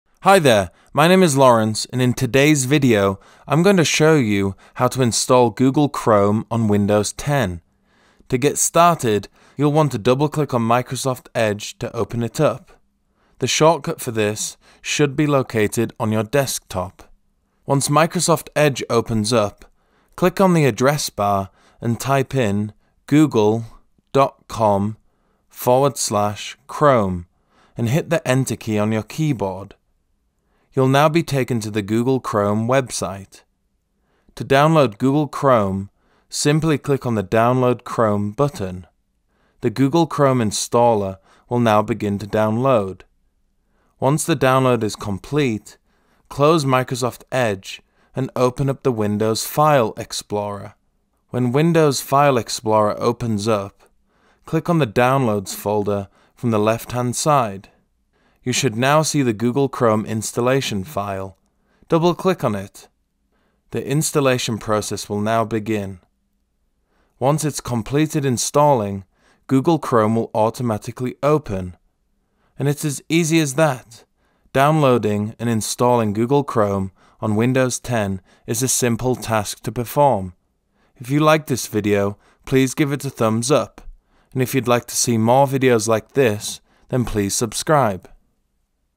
How to Install Google Chrome on Windows 10
In today’s video, I’m going to show you how to install Google Chrome on Windows 10.

1. You’ll want to double click on Microsoft Edge to open it up. The shortcut for this should be located on your desktop.
2. Once Microsoft Edge opens up, click on the address bar, and type in and hit the enter key on your keyboard.
3. You’ll now be taken to the Google Chrome website.
4. To download Google Chrome, simply click on the “Download Chrome” button.
5. The Google Chrome installer will now begin to download.
6. Once the download is complete, close Microsoft Edge, and open up the Windows File explorer.
7. When Windows File Explorer opens up, click on the “Downloads” folder from the left hand side.
8. You should now see the Google Chrome installation file, double click on it.
9. The installation process will now begin.
10. Once it’s completed installing, Google Chrome will automatically open.

And it’s as easy as that, downloading and installing Google Chrome on Windows 10 is a simple task to perform.